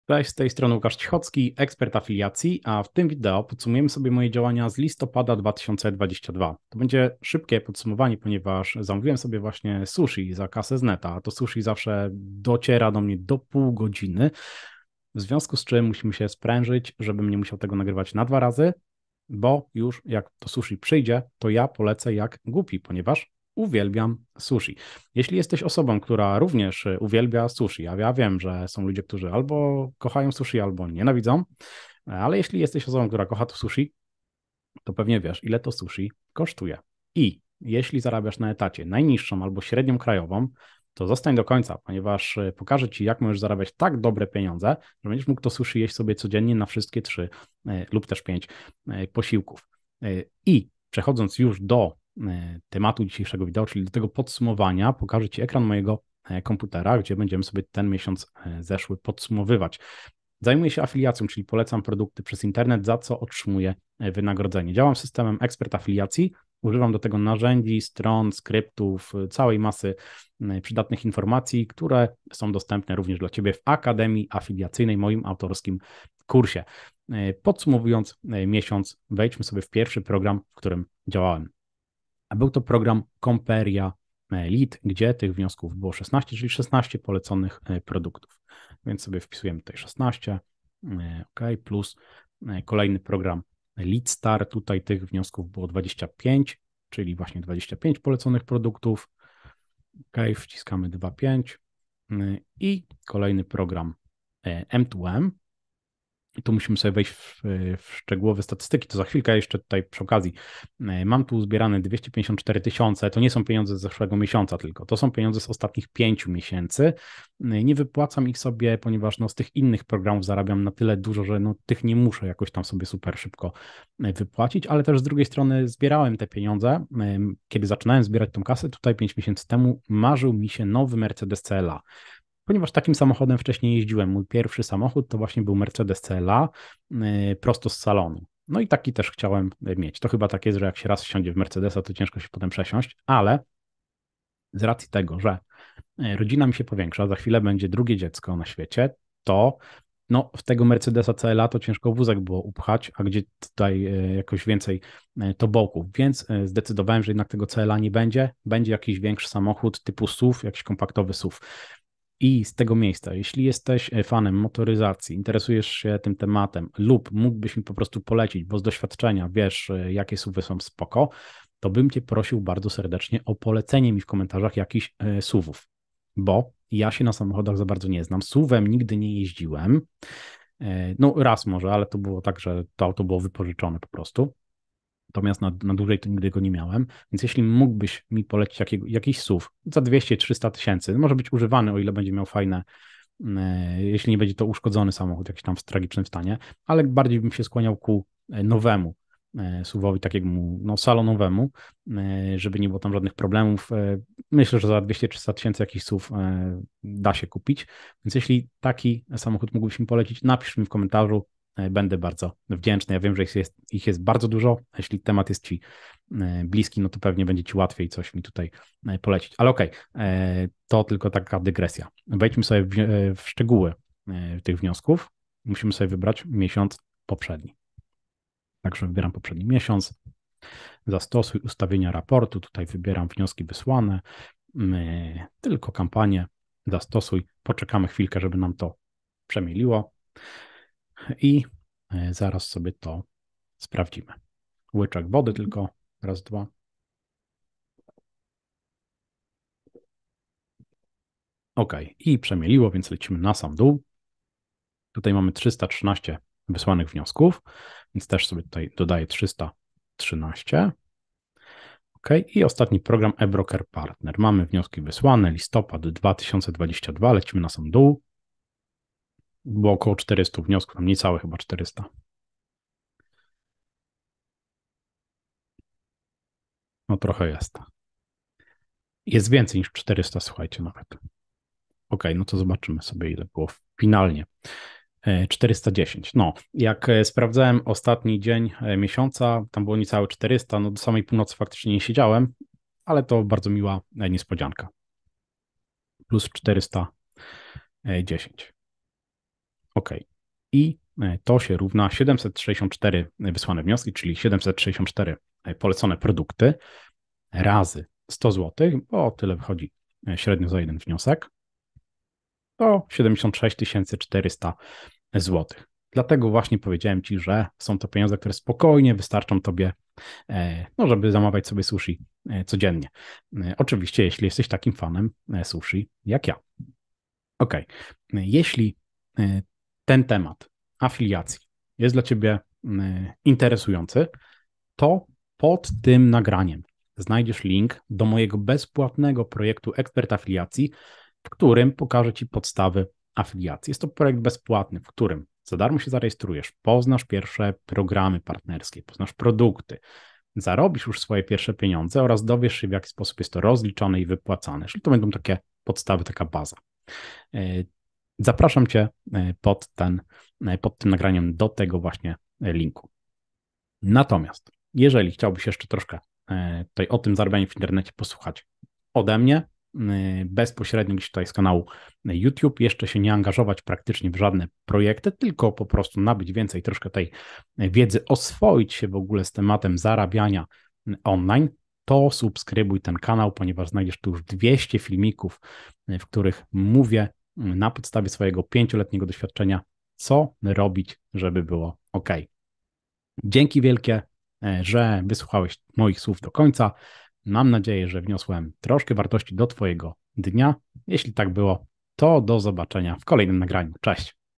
Jak W Miesiąc Zarobiłem Na Afiliacji 76 400 zł - Łukasz Cichocki M2M Ebroker Leadstar Comperialead
W tym wideo mówię o moich zarobkach z Afiliacji w listopadzie 2022